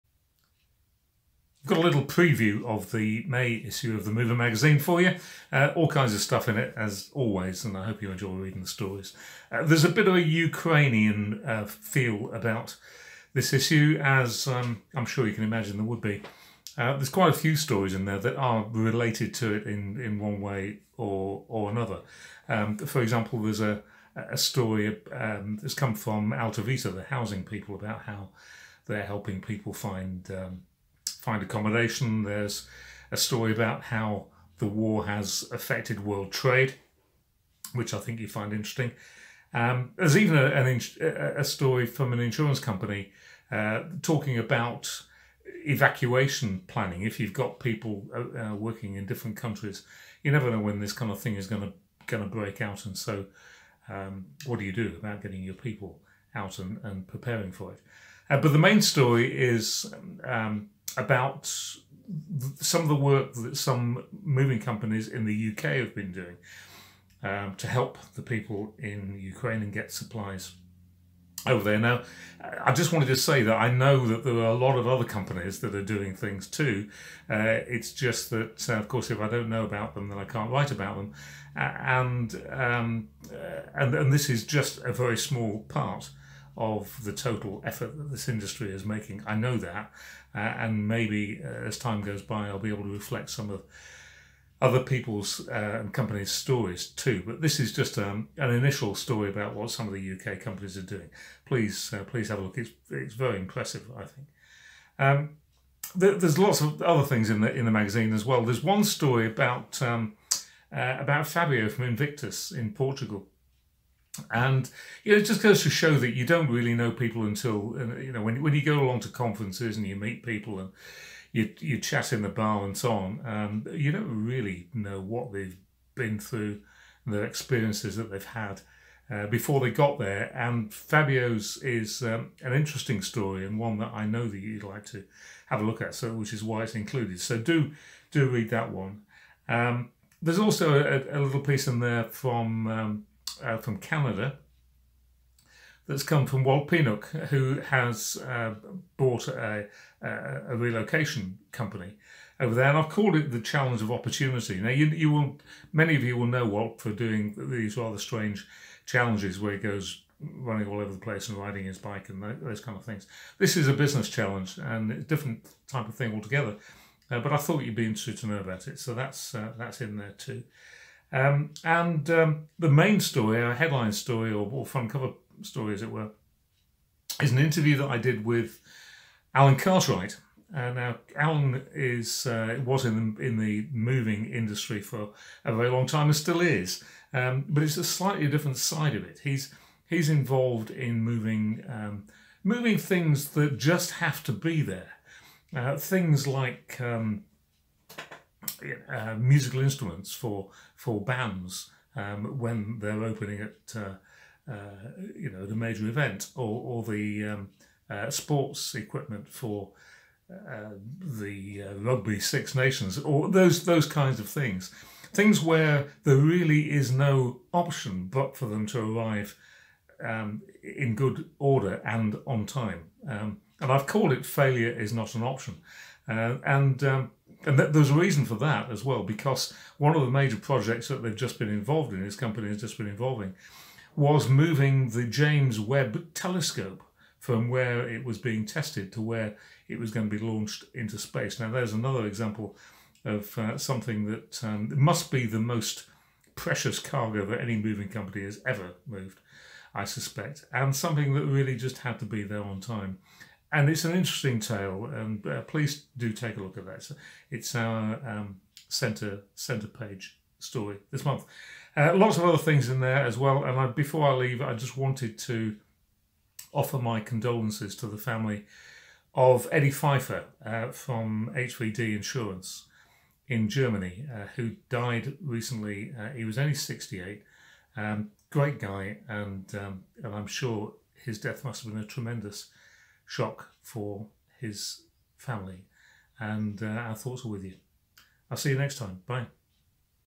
Moving industry news - The Mover May 2022
Read May's issue of The Mover to keep up to date with news, features and insights from the global moving industry